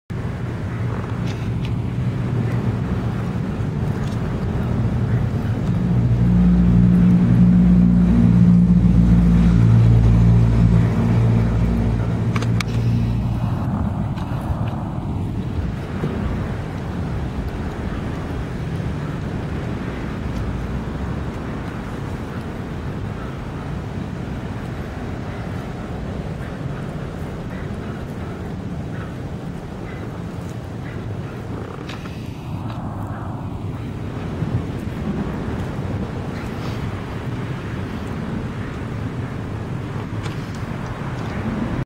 Zephyr eating lettuce
Zephyr eating lettuce, he's a greek tortoise.